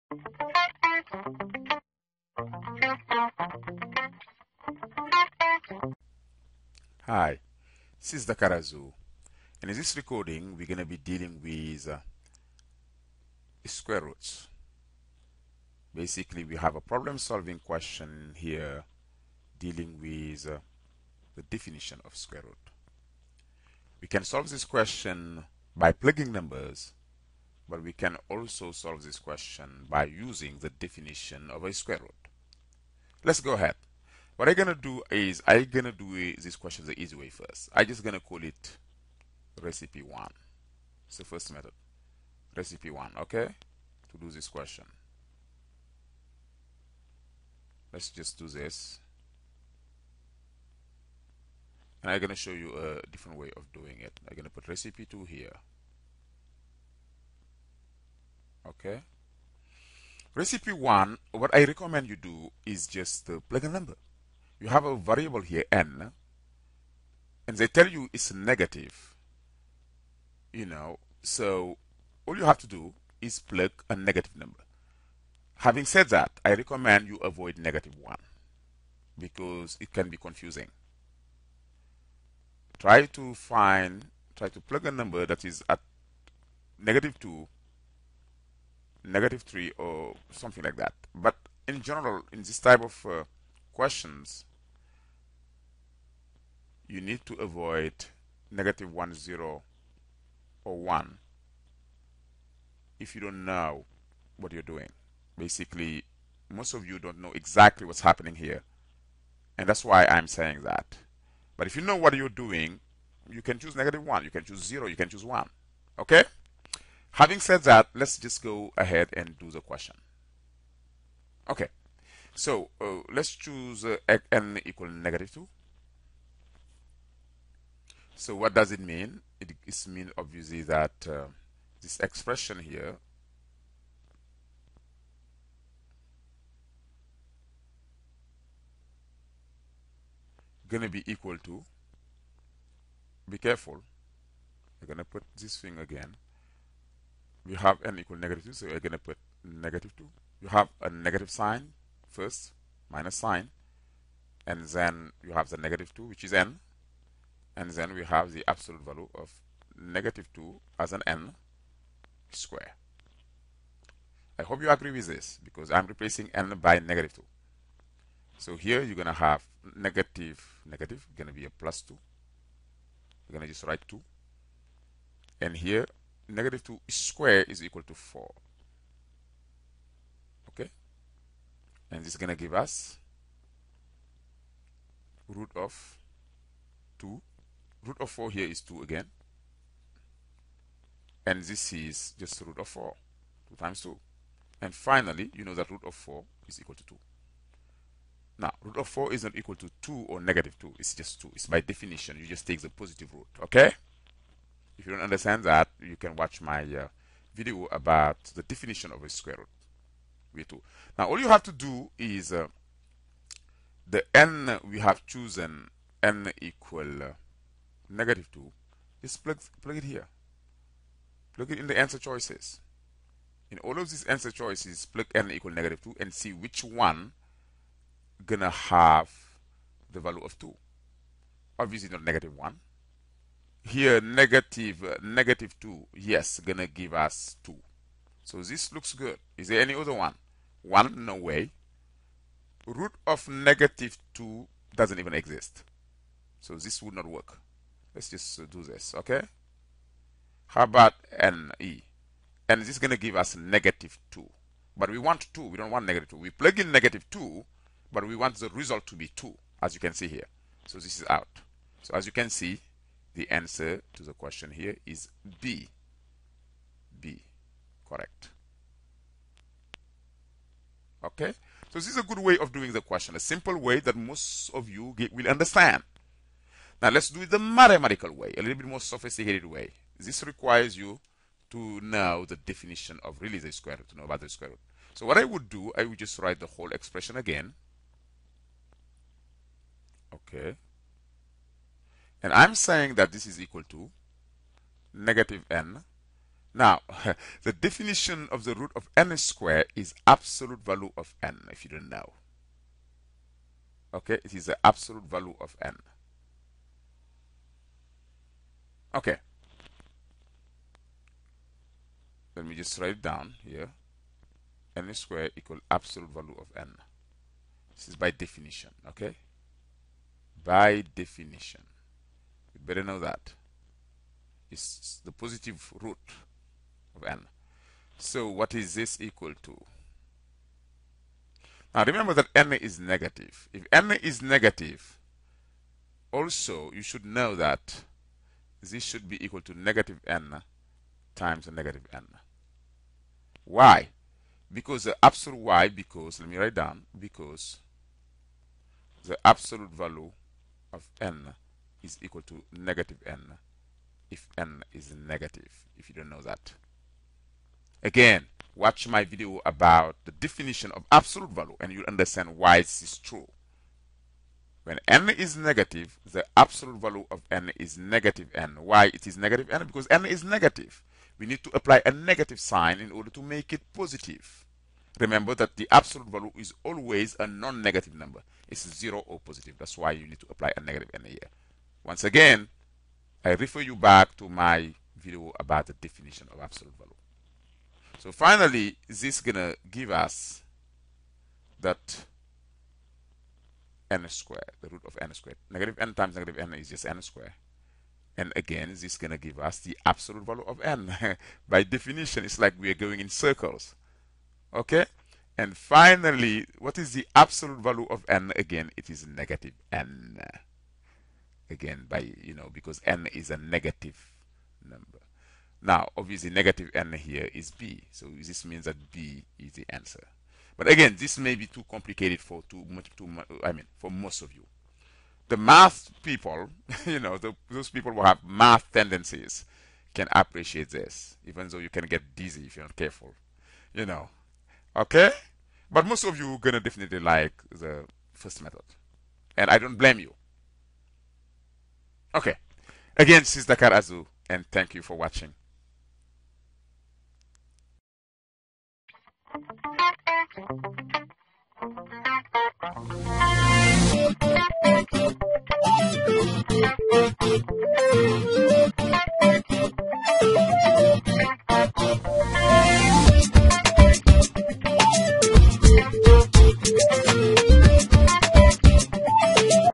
GMAT Sample Questions - Square Root - Absolute Value 3
This a GMAT Algebra Problem Solving question from GMATQuestions.org.